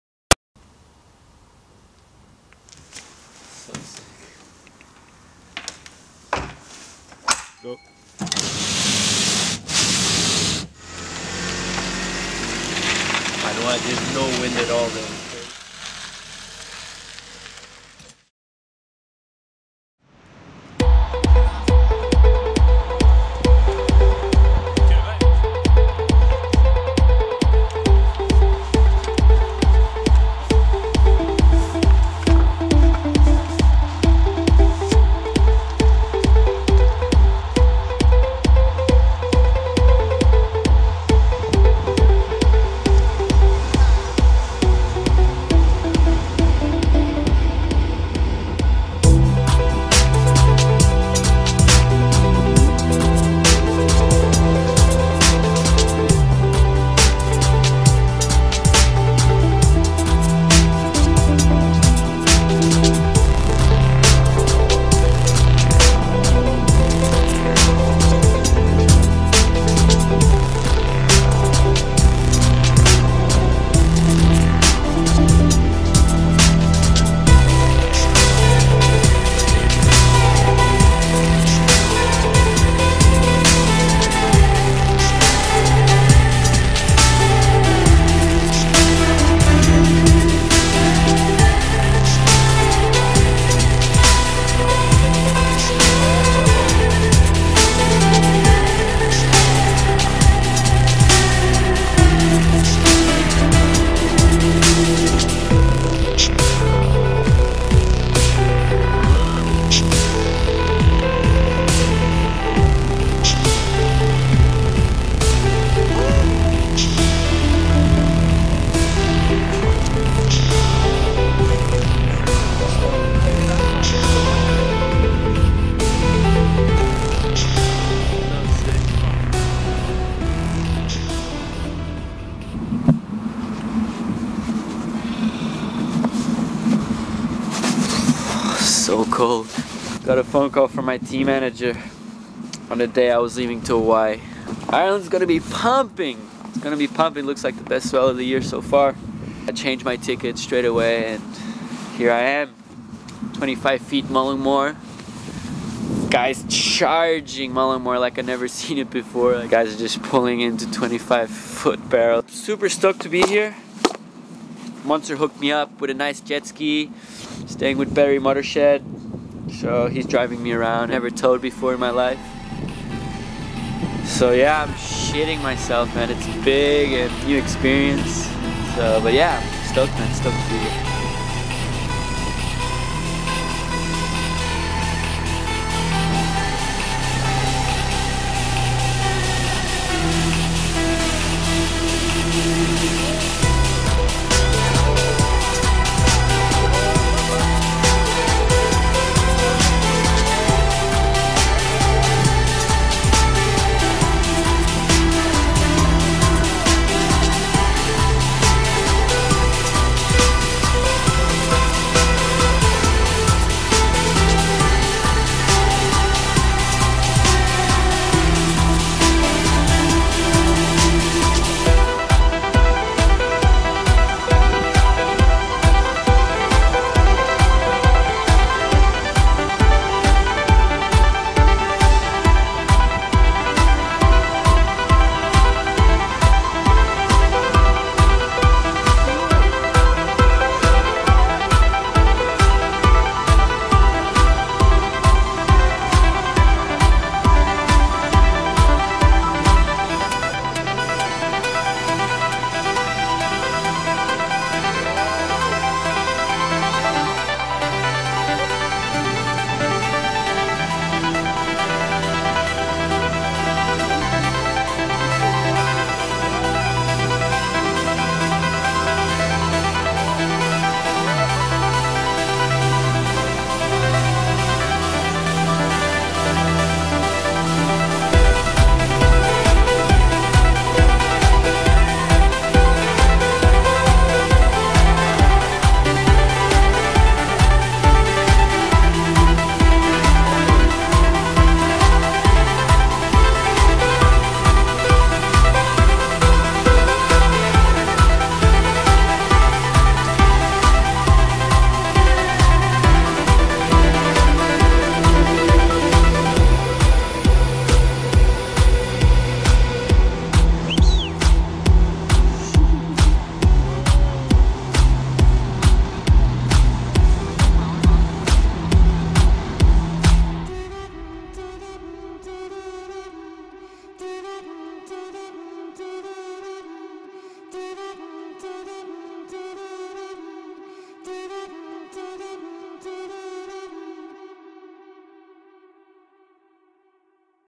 First time in Ireland - Big Wave Surfing
Swapping Hawaii for the West Coast of Ireland